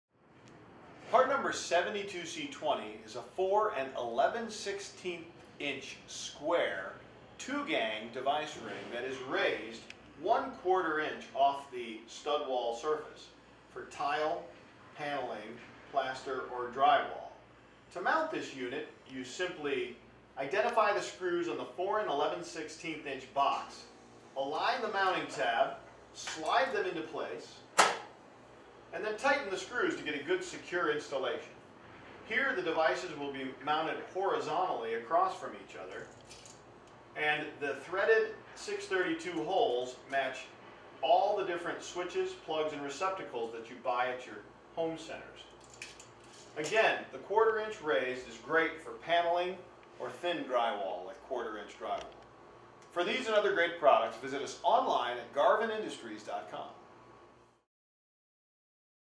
Square Box Cover: 4-11/16" Square Two Gang 1/4" Raised Device Ring 72C20
- 4-11/16" square two gang 1/4" raised device rings are used to mount switches or receptacles on junction boxes that are 1/4" deep on a finished-wall surface. The angled slots on the covers compensate for up to 12° of box misalignment on upright framing members.